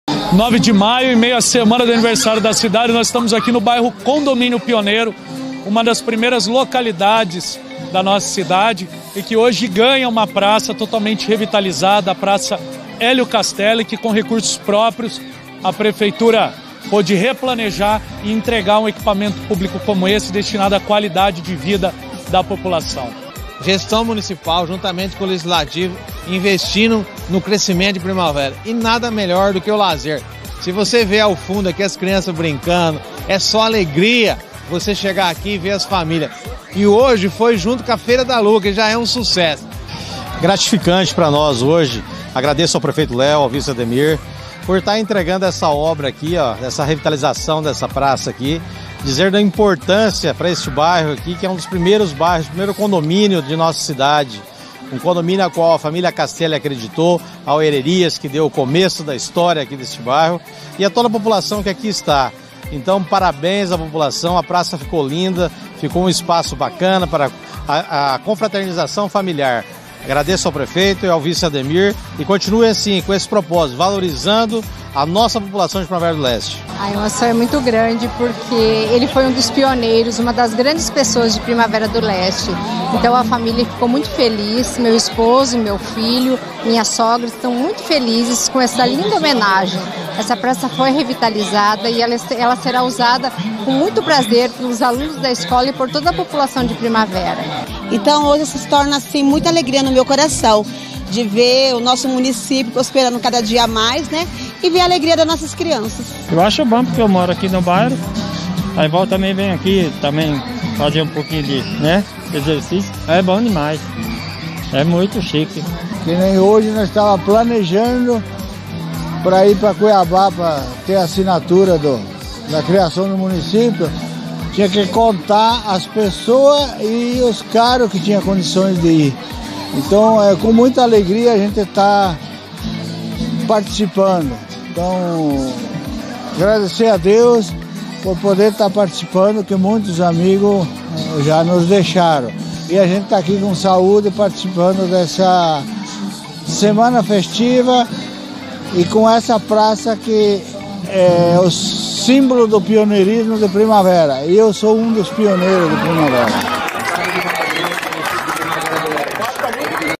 Prefeitura de Primavera de Leste reinaugura praça Élio Castelli no Condomínio Pioneiro
A Prefeitura de Primavera do Leste reinaugurou a praça Élio Castelli, no condomínio Pioneiro.

A solenidade aconteceu por volta das 18h, além da participação de moradores do bairro, familiares do homenageado também compareceram.